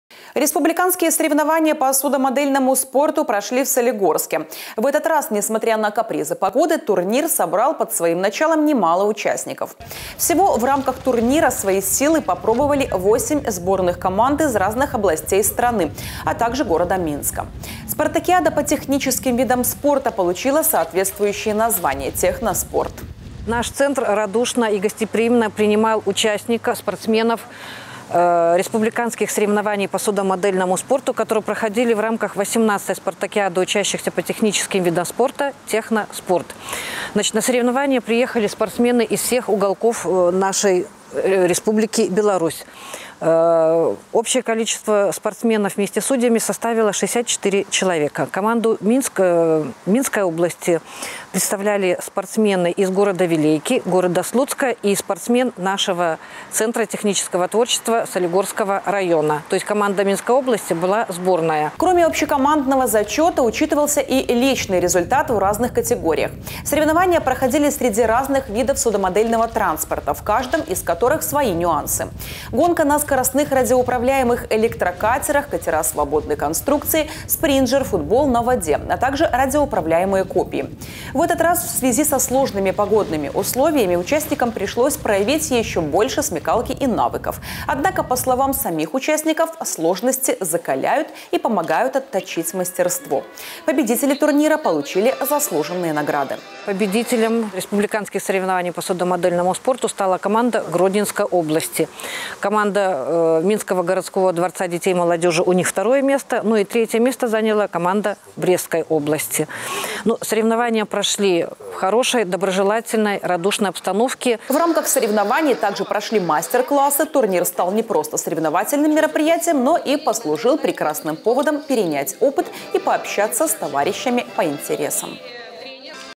Республиканский турнир «Техно-спорт»
Республиканские соревнования по судомодельному спорту прошли в Солигорске. В этот раз несмотря на капризы природы турнир собрал под своим началом не мало участников. Спартакиада по техническим видам спорта получила  соответствующее название - «Техно спорт».